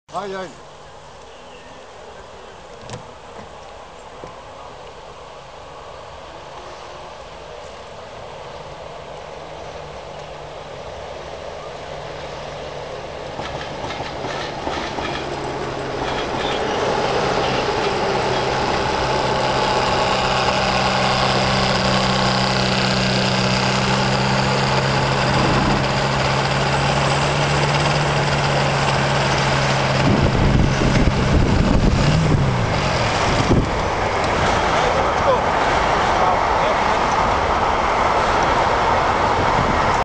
Prvi let na zmaju Tihomira Radovanovica
Prvi let Tihog na zmaju sa instruktorom letenja Vladimirom Stojkovicem